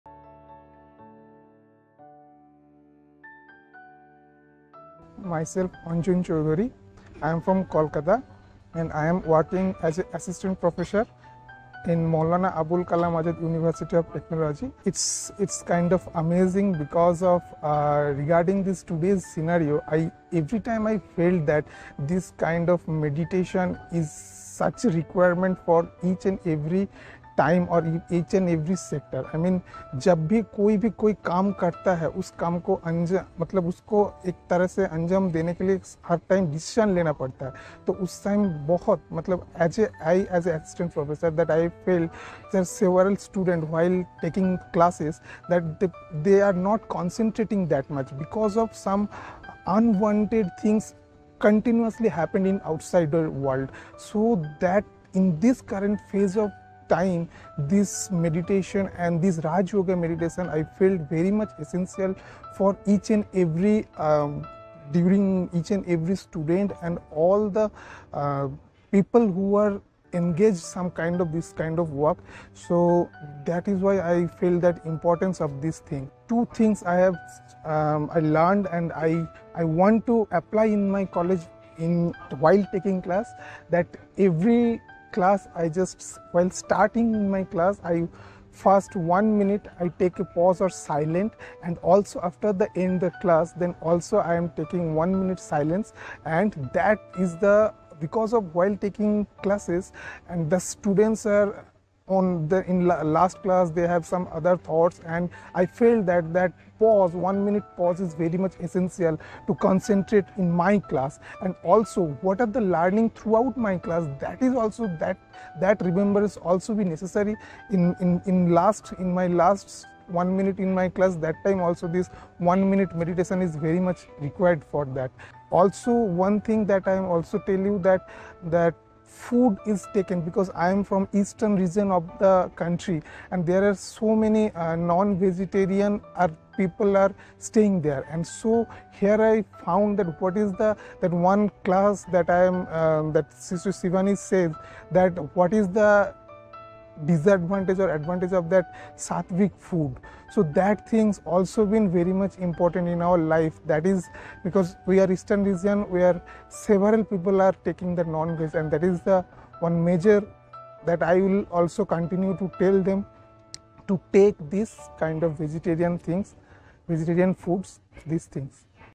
Anjan Choudhary || Asst. Professor, MAKAUT @ West Bengal
Experience_Sharing by delegates of Inner_Technology National Conference: "Inner_Resilience".